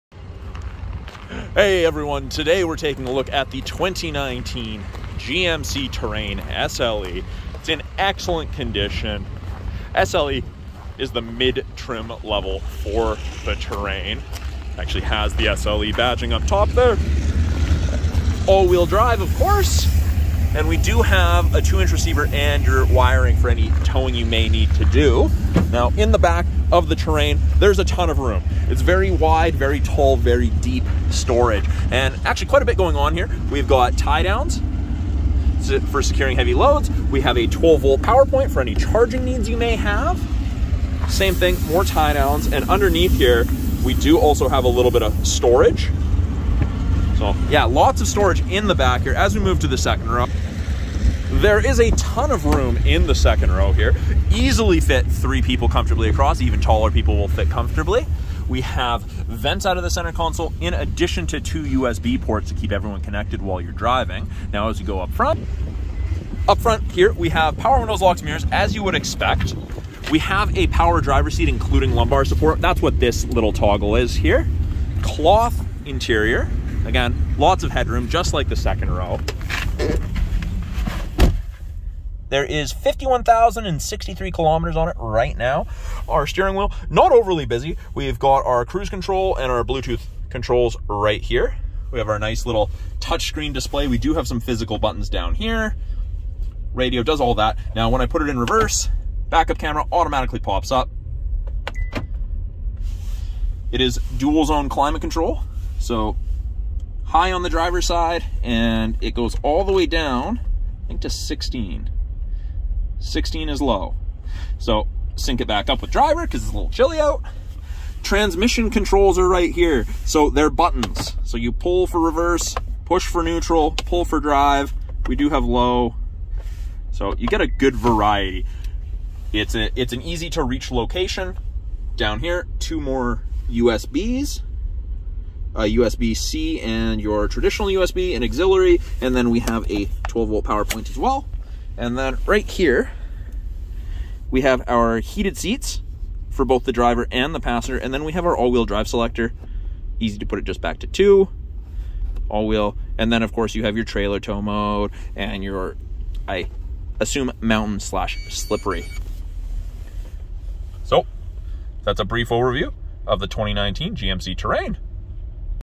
2019 GMC TERRAIN SLE | Stock# 21CT599710
2019 GMC TERRAIN SLE | Stock# 21CT599710

Come to Subaru of Prince George for a test drive of this 2019 GMC TERRAIN SLE. This unit has 4 doors and seating for 5. It comes with 51,035 kms and is an All-Wheel Drive SUV. We look forward to helping you find the perfect New or Pre-Owned model here at Subaru of Prince George.

Climate Control
Multi-Zone A/C
A/C
Security System
AM/FM Stereo
ABS
4-Wheel Disc Brakes
Cruise Control
Floor Mats
Daytime Running Lights
Keyless Entry
Power Door Locks
Heated Mirrors
Power Mirror(s)
Cloth Seats
Bucket Seats
Temporary Spare Tire
Traction Control
Aluminum Wheels
Power Windows
MP3 Player
Heated Front Seat(s)
Privacy Glass
Steering Wheel Audio Controls
Power Outlet
HID headlights
Automatic Headlights
Trip Computer
Bluetooth Connection
Telematics
Remote Engine Start
Back-Up Camera
Stability Control
Keyless Start
WiFi Hotspot

You can test drive this unit at Subaru of Prince George by calling or stop by to view this model.

105 Brunswick St, Prince George, BC V2L 2B2